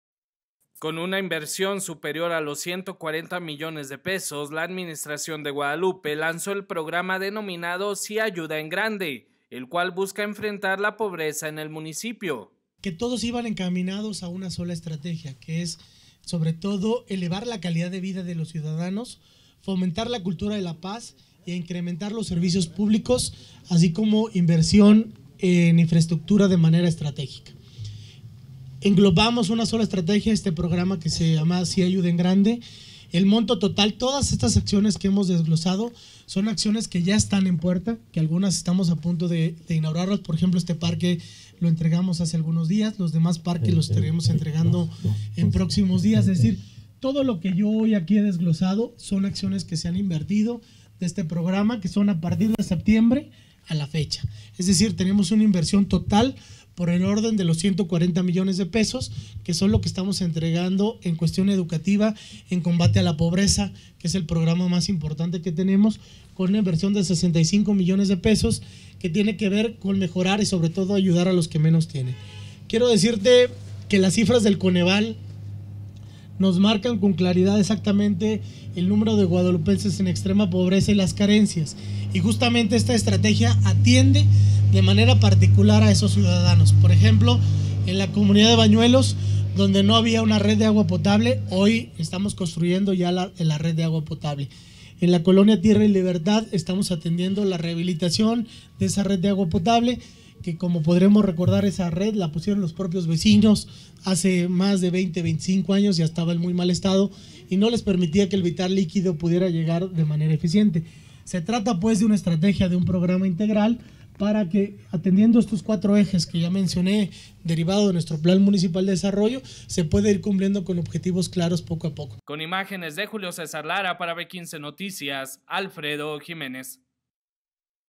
AYUNTAMIENTO DE GUADALUPE LANZA PROGRAMA "SI AYUDA EN GRANDE"
Con una inversión superior a los 140 millones de pesos, la administración de Guadalupe, lanzó el programa denominado Sí Ayuda en Grande, el cual busca enfrentar la pobreza en el municipio.